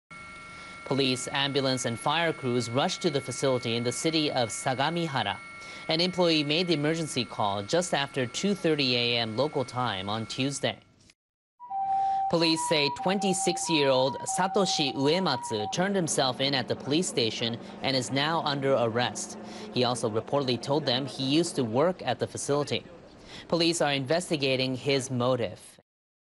Raw: At Least 15 Killed in Knife Attack in Japan
Japan's national broadcaster NHK is reporting that 15 people were killed and 45 injured in a knife attack at a facility for the handicapped in Sagamihara, just outside Tokyo. (July 25)